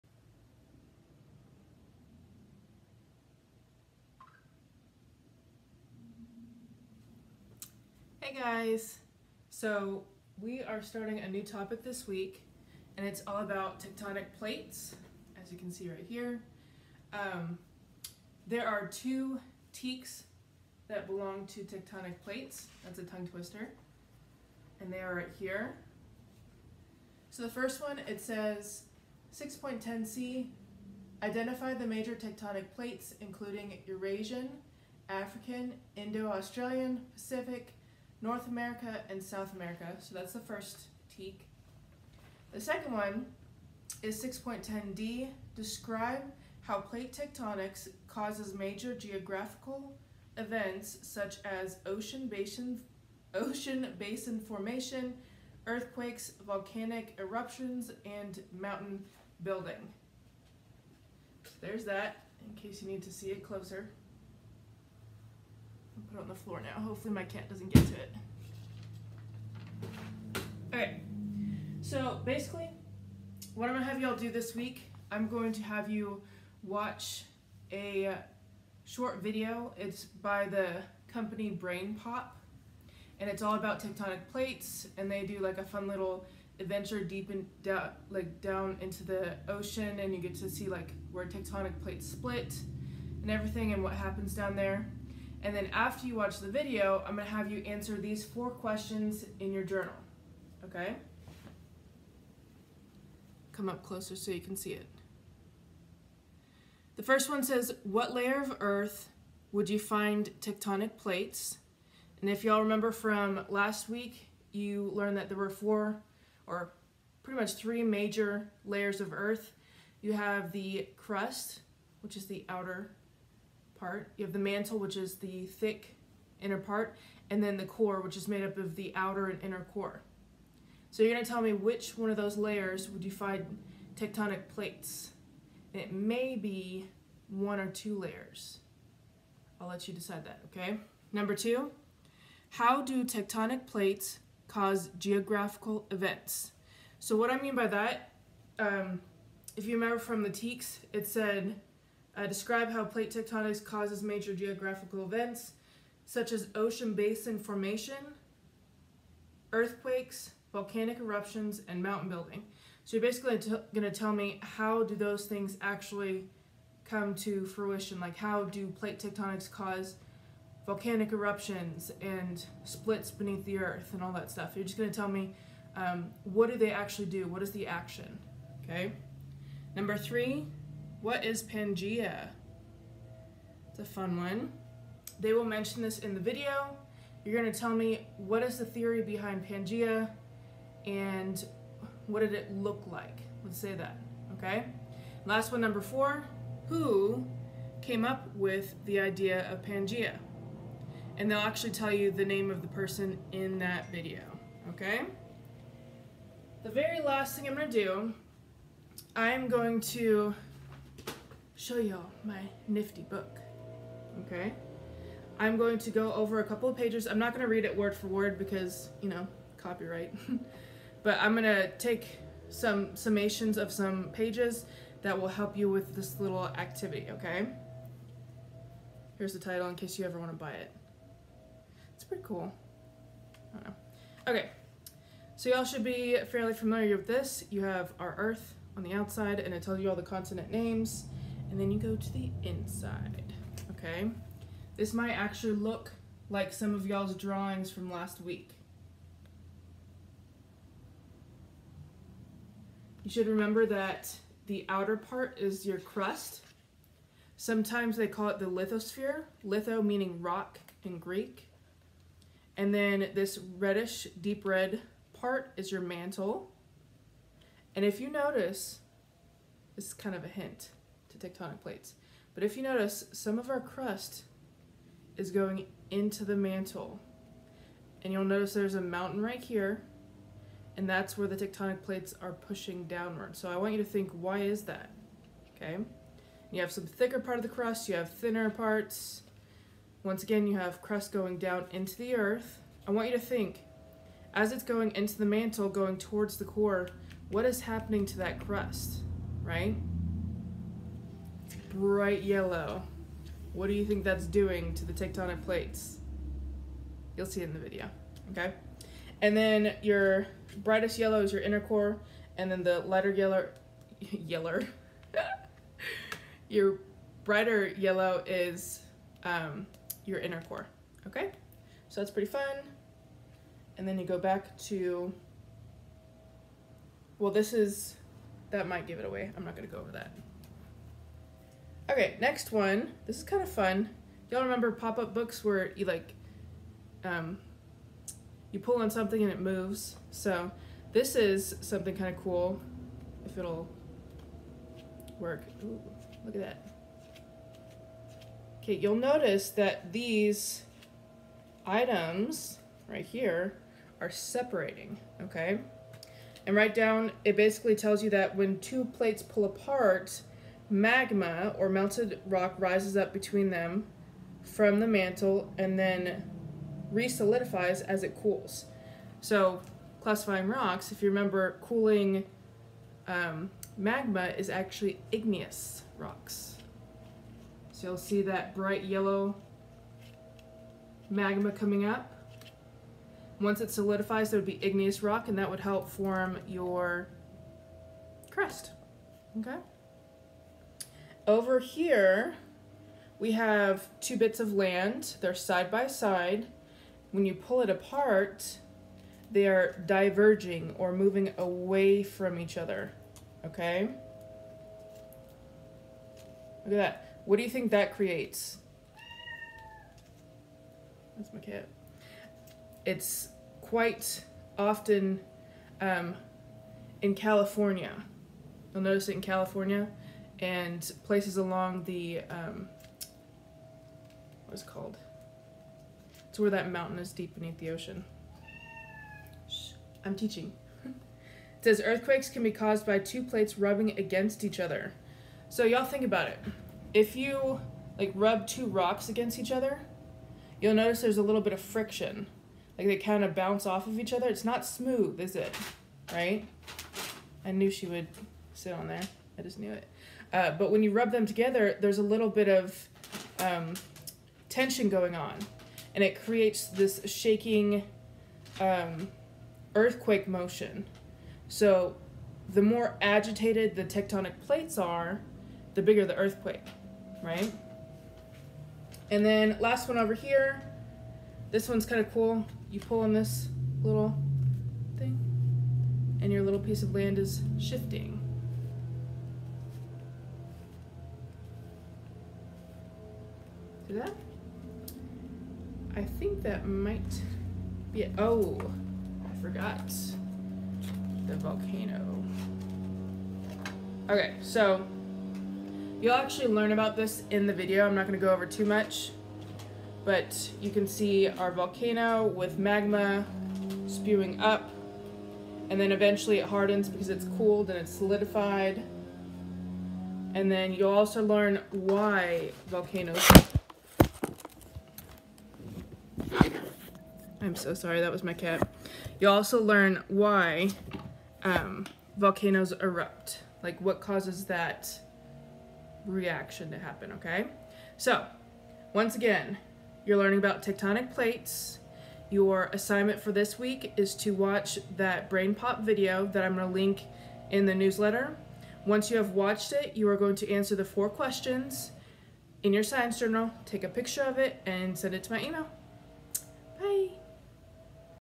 Lesson 3: Tectonic Plates
In this video, you will learn a little more about tectonic plates and why they are so important!